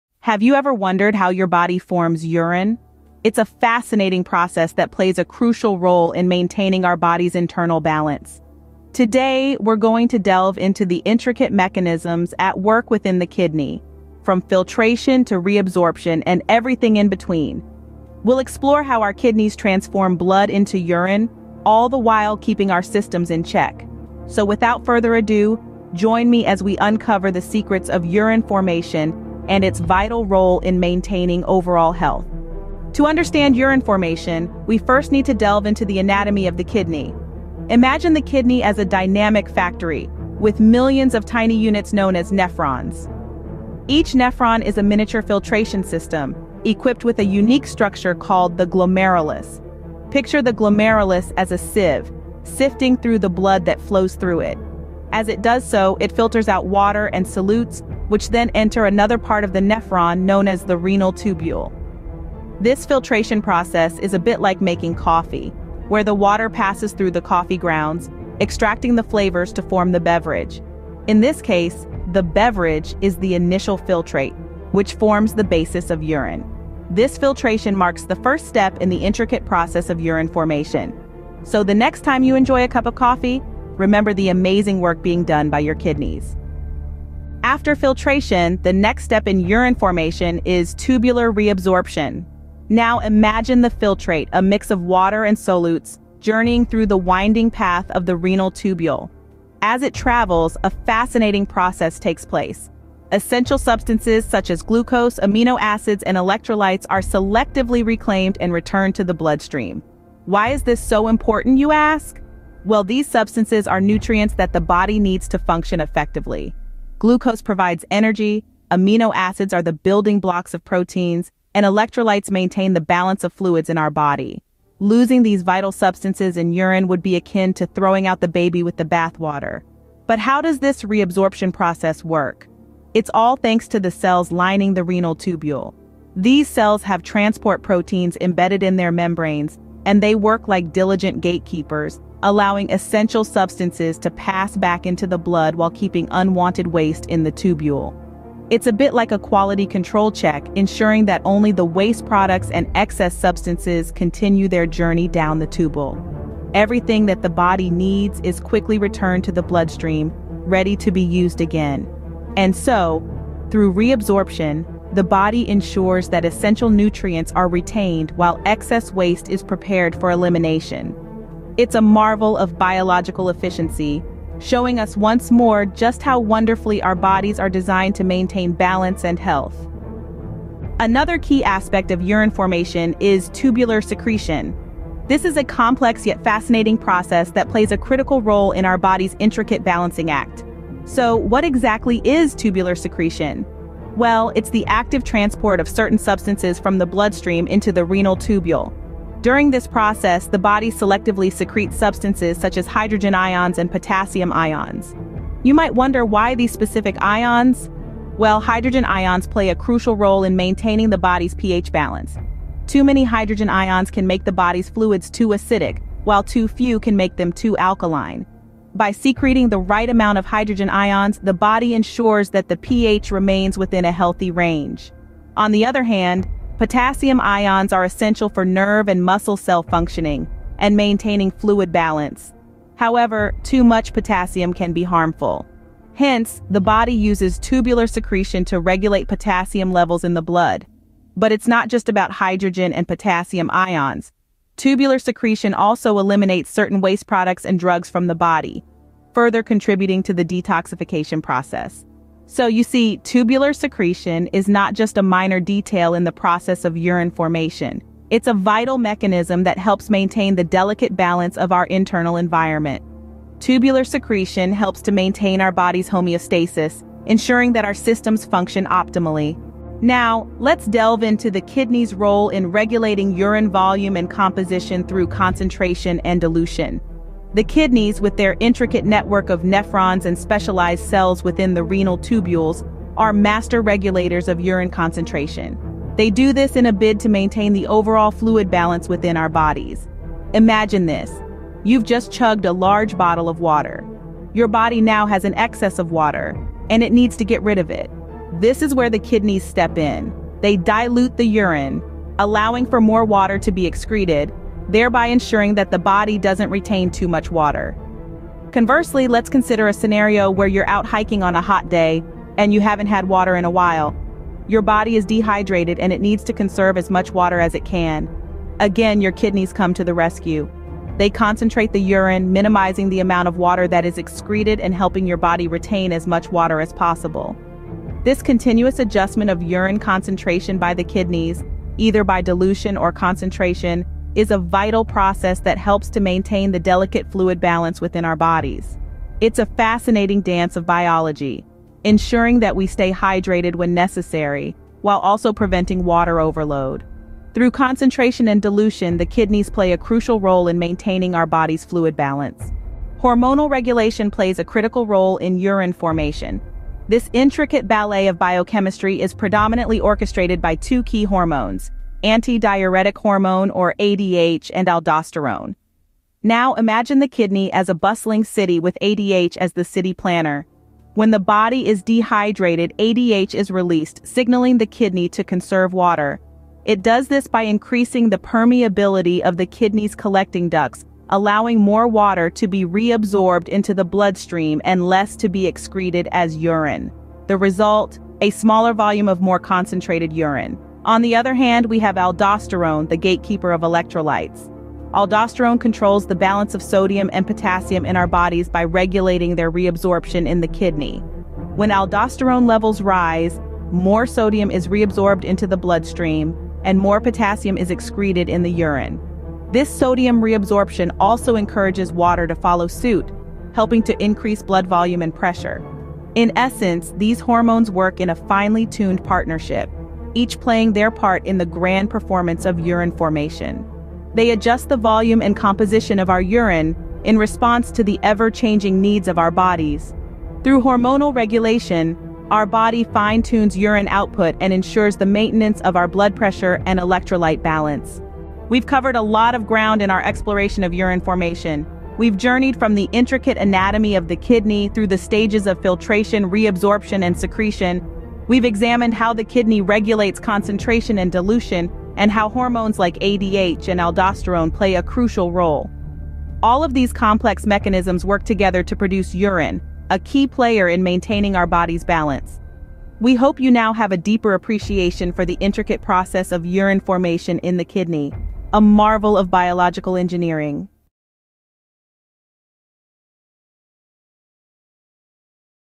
Mastering Urine Formation: Unraveling the Kidney's Secret Process
Dive deep into the fascinating world of urine formation in the kidney! 🌊💧 In this comprehensive video, we unravel the intricate process behind how urine is made, exploring filtration, reabsorption, and hormonal regulation. Join us as we uncover the secrets of kidney function and its vital role in maintaining overall health and homeostasis.

**Video Segment Breakdown:**

1. Introduction (0:00 - 0:30) - Overview of the video and introduction to urine formation in the kidney.
2. Kidney Anatomy and Filtration (0:30 - 1:30) - Explanation of the kidney's anatomy and the initial filtration process.
3. Tubular Reabsorption (1:30 - 3:00) - Detailed exploration of tubular reabsorption and its role in retaining essential nutrients.
4. Tubular Secretion (3:00 - 4:30) - Discussion on tubular secretion and its importance in maintaining electrolyte balance.
5. Concentration and Dilution (4:30 - 6:00) - Explanation of how the kidney regulates urine concentration and volume.
6. Hormonal Regulation (6:00 - 7:30) - Insight into hormonal regulation and its impact on urine formation.
7. Conclusion (7:30 - 8:00) - Recap of key points and the importance of kidney function for overall health.